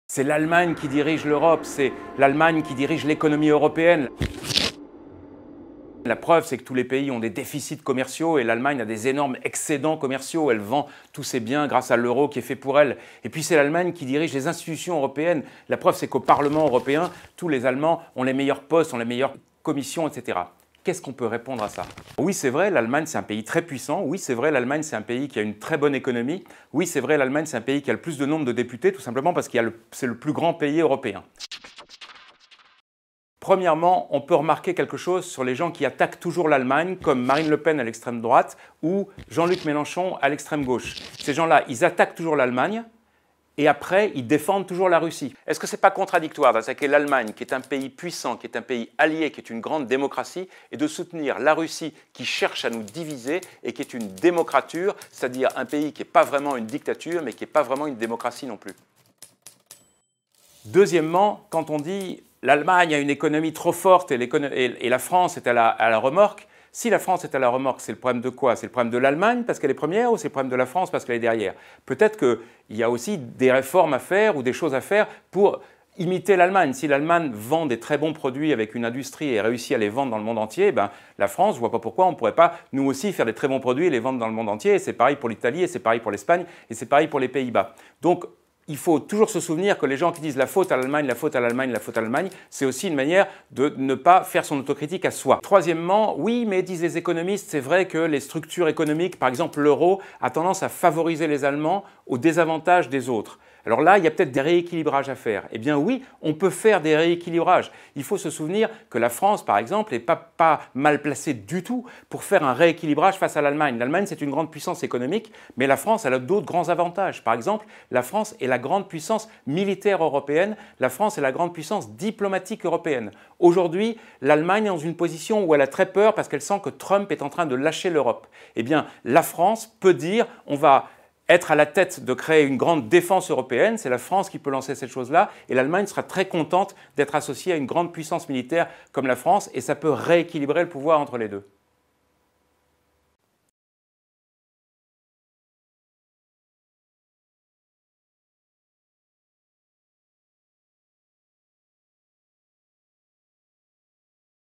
Peurs sur l'Europe #2 : "On est dirigés par l'Allemagne"
Pendant toute la campagne des élections européennes du 26 mai prochain, notre journaliste François Reynaert, alias "Oncle Obs", scrute les peurs et les angoisses des électeurs face à l'Union. Dans ce deuxième numéro, la question de la prépondérance allemande.

Sujet : François Reynaert
Réalisation : Mélody Locard
Direction artistique : Jade Raoulx